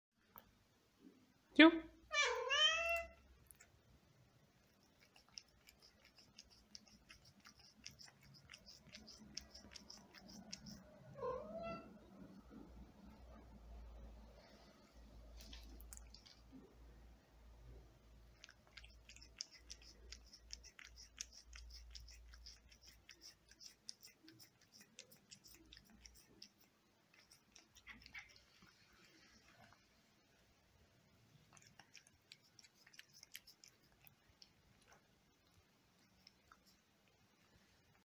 CAT EATS GRAPES (asmr)
Caju eats grape
Nikus wants some

Ju&Nikus - Cat Life